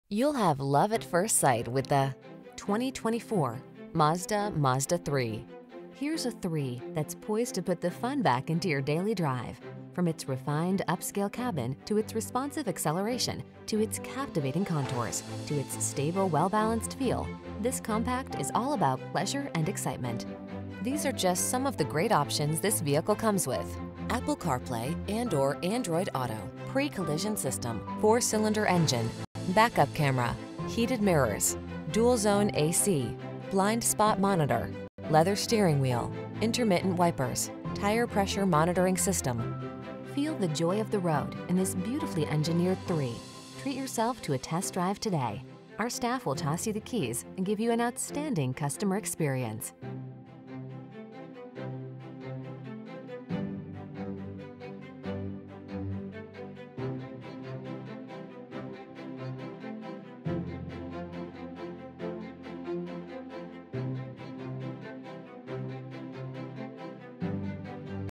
2024 Mazda Mazda3_Sedan 2.5 S Select Sport AZ Phoenix, Chandler, Gilbert, Tempe, Maricopa
New 2024 Mazda Mazda3_Sedan 2.5 S Select Sport available at Horne Mazda located in 7777 South Test Drive, Tempe, AZ, 85284. Servicing the Phoenix, Chandler, Gilbert, Tempe, Maricopa area.

You'll have love at first sight with the   2024  MAZDA Mazda3. Here's a 3 that's poised to put the fun back into your daily drive. From its refined, upscale cabin, to its responsive acceleration, to its captivating contours, to its stable, well-balanced feel, this compact is all about pleasure and excitement. These are just some of the great options this vehicle comes with: Apple Carplay®/Android Auto®, Pre-Collision System, 4 Cylinder Engine, Back-Up Camera, Heated Mirrors, Dual Zone A/C, Blind Spot Monitor, Leather Steering Wheel, Intermittent Wipers, Tire Pressure Monitoring System. Feel the joy of the road in this beautifully engineered 3. Treat yourself to a test drive today. Our staff will toss you the keys and give you an outstanding customer experience.